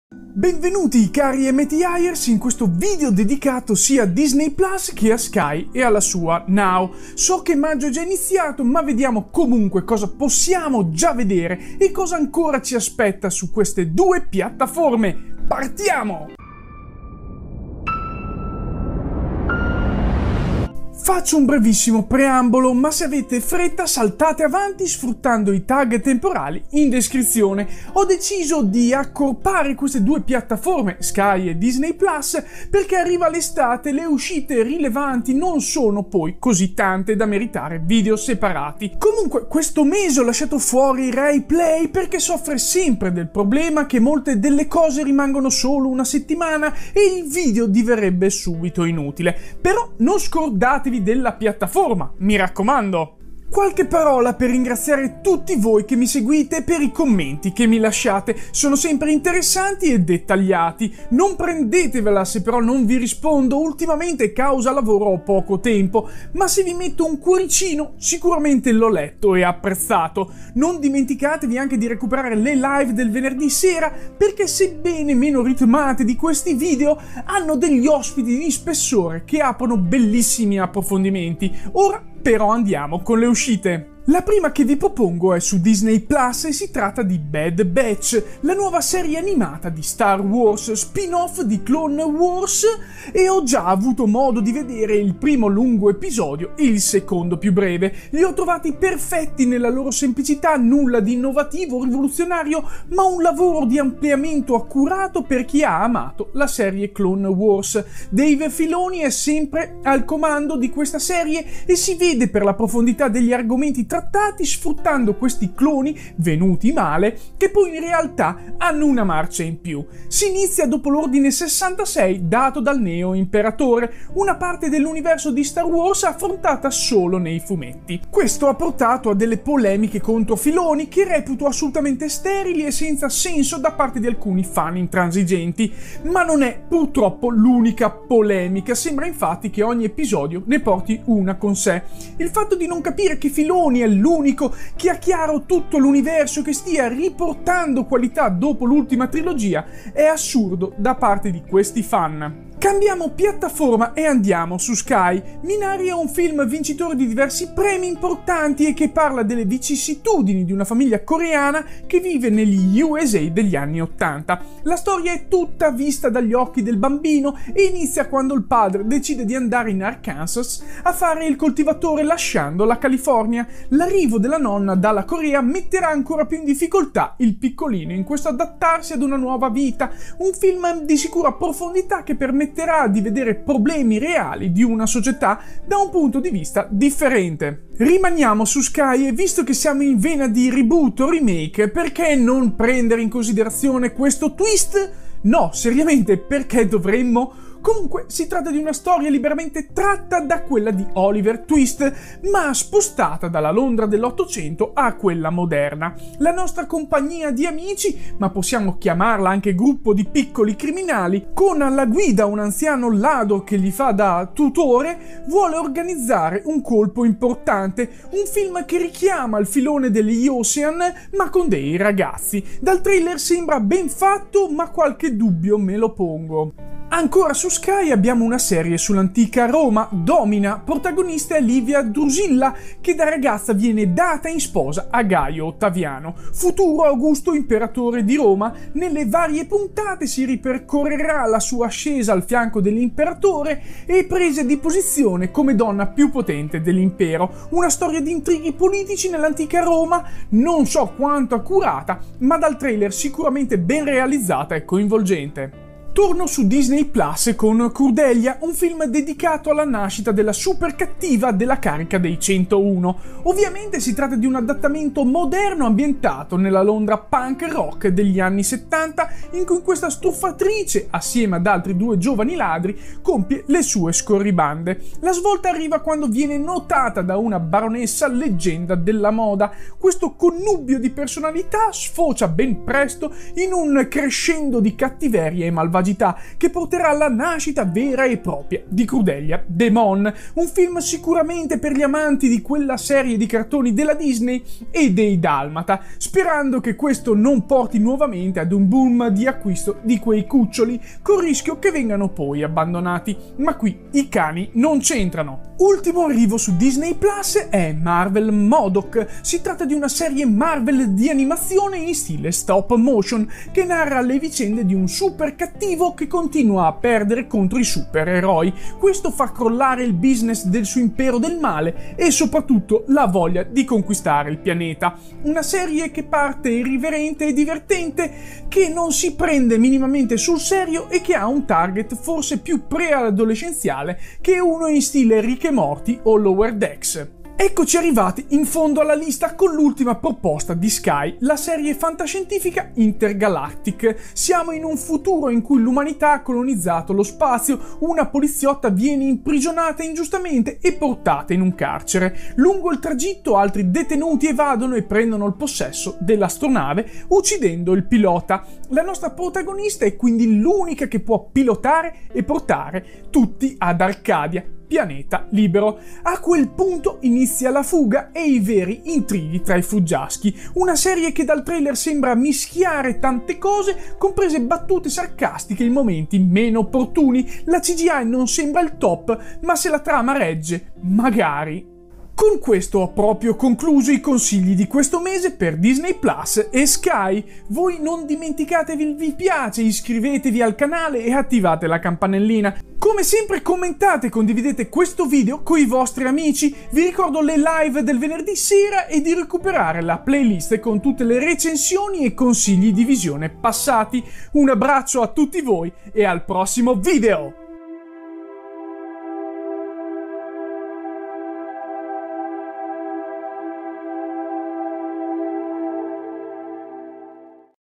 7 Film o Serie da Guardare su Disney Plus o Sky a Maggio 2021
Anche se Maggio è già iniziato da un po', date un'occhiata a quello che si può vedere questo mese

| Argomenti |
0:00 Introduzione
0:20 Un breve preambolo
0:53 Recuperate le live
1:23 The Bad Batch
2:32 Minari
3:07 Twist
3:46 Domina
4:15 Crudelia
5:05 Marvel’s M.O.D.O.K.
5:36 Intergalactic
6:21 Gli altri consigli